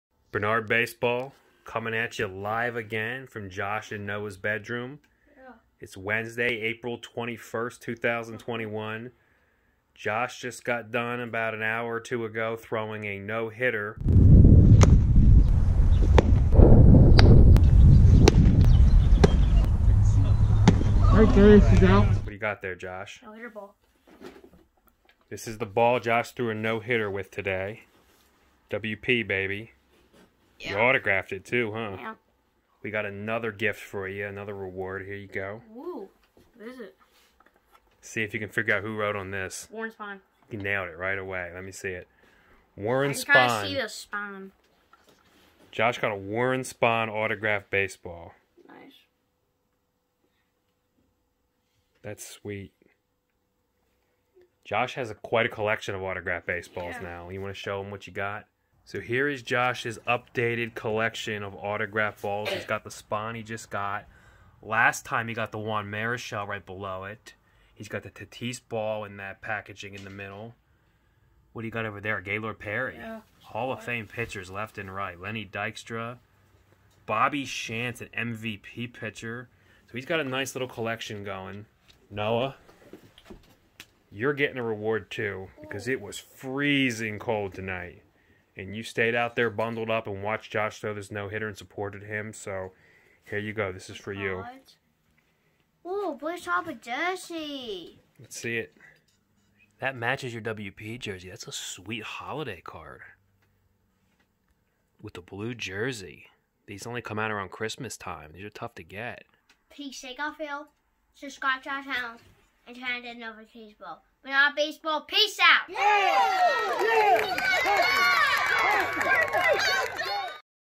JOSH GETS A WARREN SPAHN AUTOGRAPHED BASEBALL
Josh earns another reward after firing a no hitter!  4/21/2021.